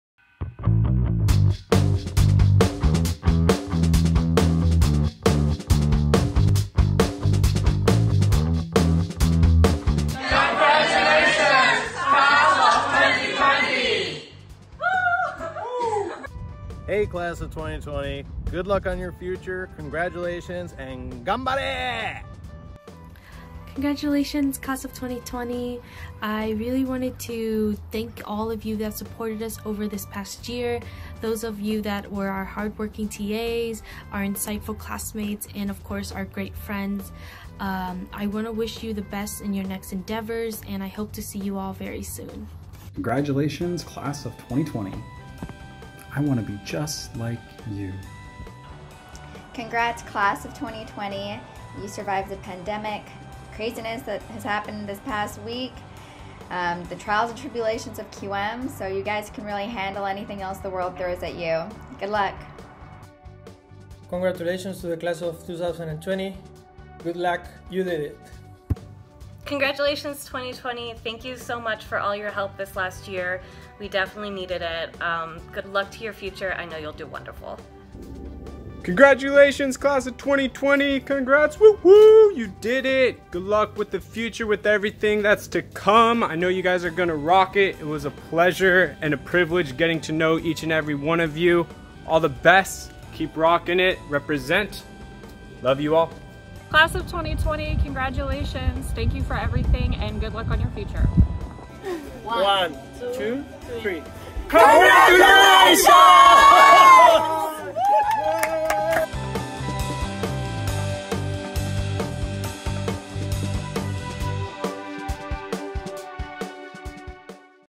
GPS Commencement 2020: Class of 2021 Well Wishes
June 13, 2020: School of Global Policy and Strategy 2020 Commencement Class of 2021 Well Wishes